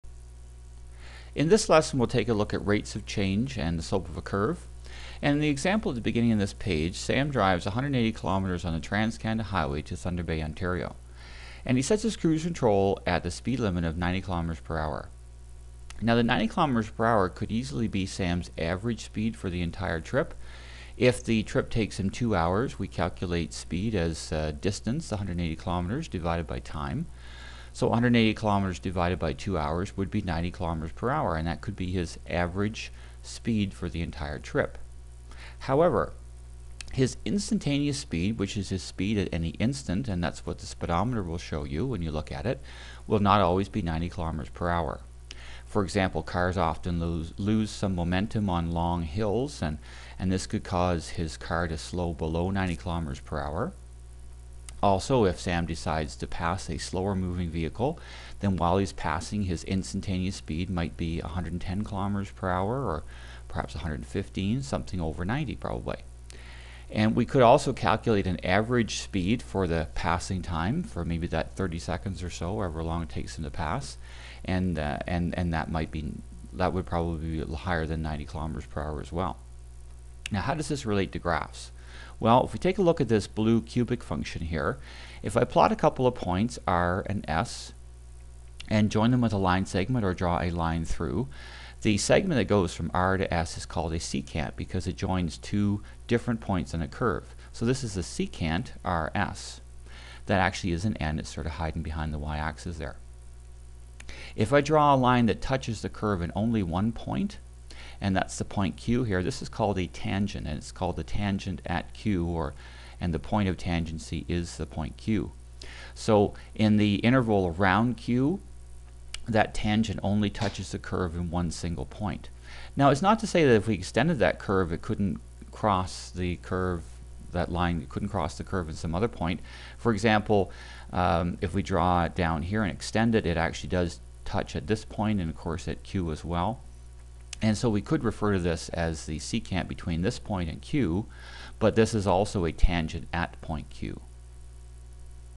Rates of Change and the Slope of a Curve Part 1
This lesson discusses average and instantaneous rates of change and shows how to calculate these rates. This is the first part of a two part lesson.  This lesson was created for the Calculus and Vectors (MCV4U) course in the province of Ontario, Canada.